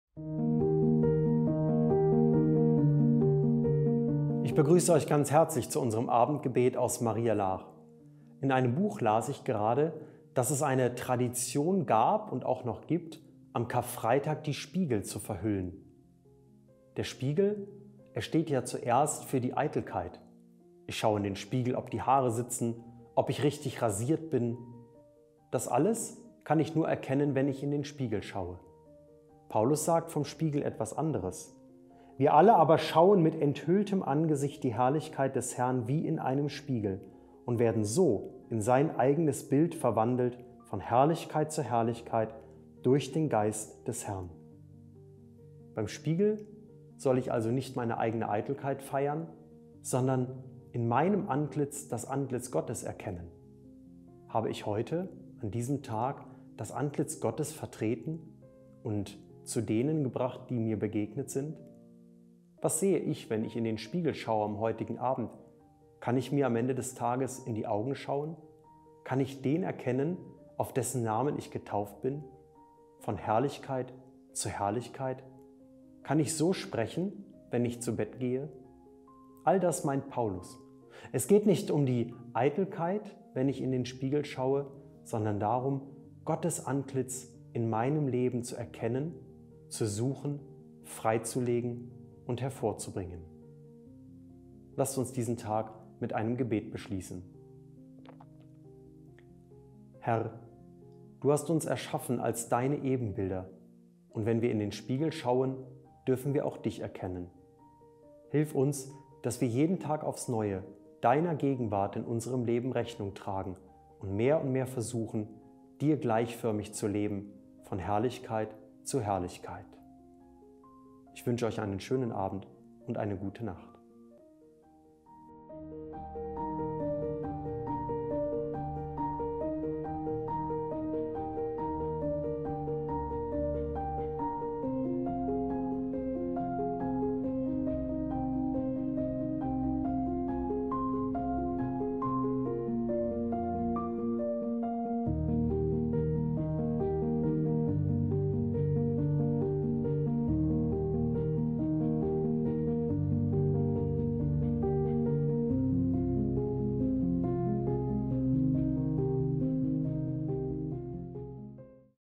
Abendgebet aus Maria Laach 13. Juni 2023
Schauen wir nicht nur in den Spiegel, um unsere Äußerlichkeiten zu bewundern. Lassen wir uns stattdessen von Paulus inspirieren und erkennen das wahrhaftige Antlitz Gottes in uns (vgl. 2 Kor 3,18).